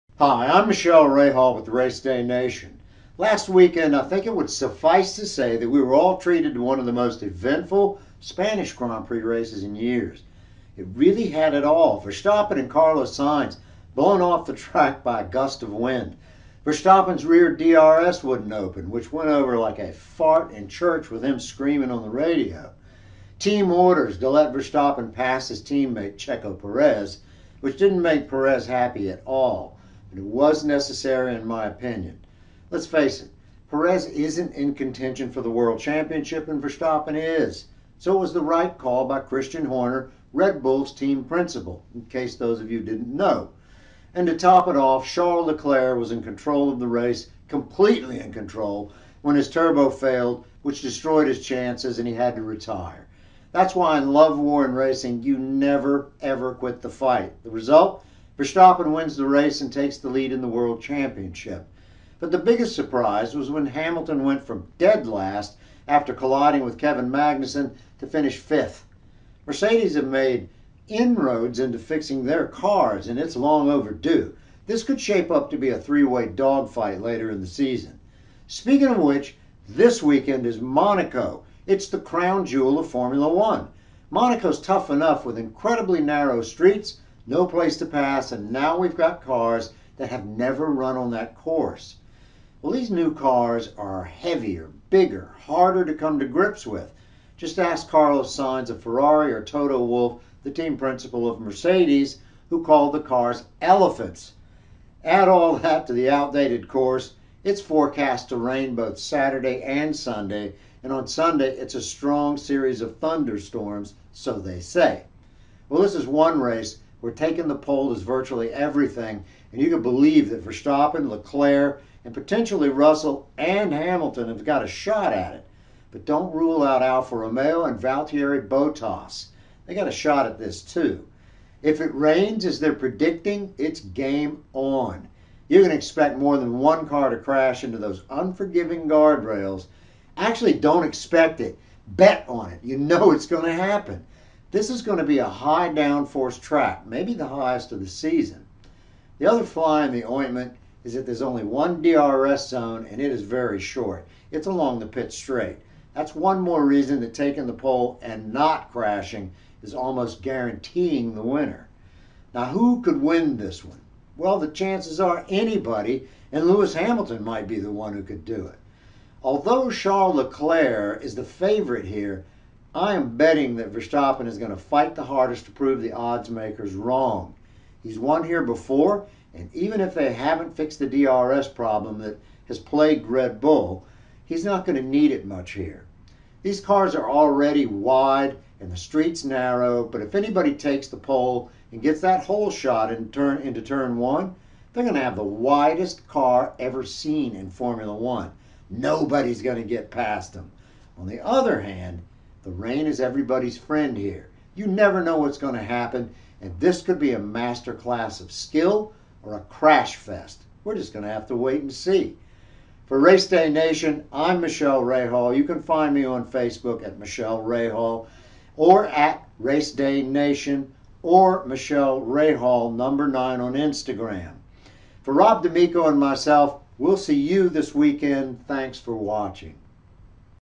Formula1 Spanish GP results and Monaco GP predictions
Michele Rahal breaks down the results from the Spanish GP and how Max Verstappen passes teammate to win and how Mercedes updates made a diference for both George Russell and Lewis Hamiltion. Plus, what took Ferrari out of contention for the win.